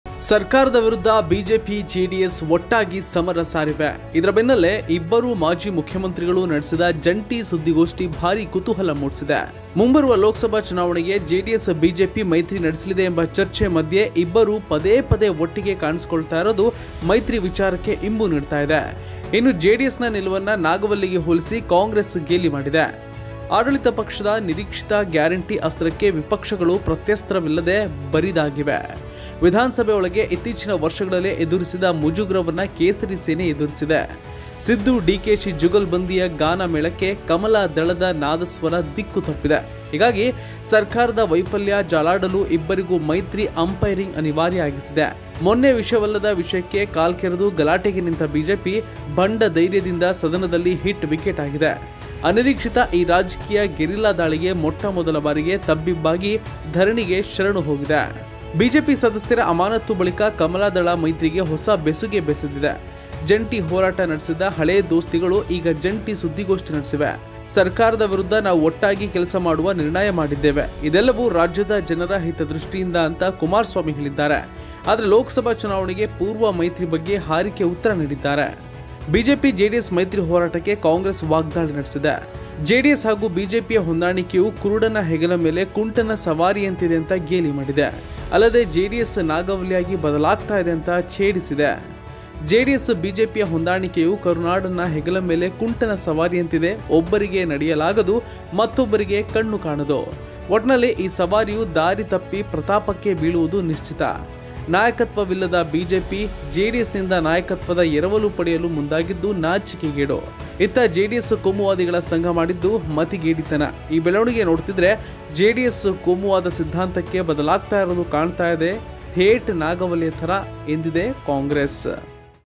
Basavraj Bommai Kumarswamy : ಬೊಮ್ಮಾಯಿ-ಹೆಚ್‌ಡಿಕೆ ಒಗ್ಗಟ್ಟಿನ ಹೋರಾಟ!
ಸರ್ಕಾರದ ವಿರುದ್ಧ ಬಿಜೆಪಿ-ಜೆಡಿಎಸ್ ಒಟ್ಟಾಗಿ ಸಮರ ಸಾರಿವೆ. ಇದರ ಬೆನ್ನಲ್ಲೇ ಇಬ್ಬರು ಮಾಜಿ ಮುಖ್ಯಮಂತ್ರಿಗಳು ನಡೆಸಿದ ಜಂಟಿ ಸುದ್ದಿಗೋಷ್ಠಿ ಭಾರೀ ಕುತೂಹಲ ಮೂಡಿಸಿದೆ.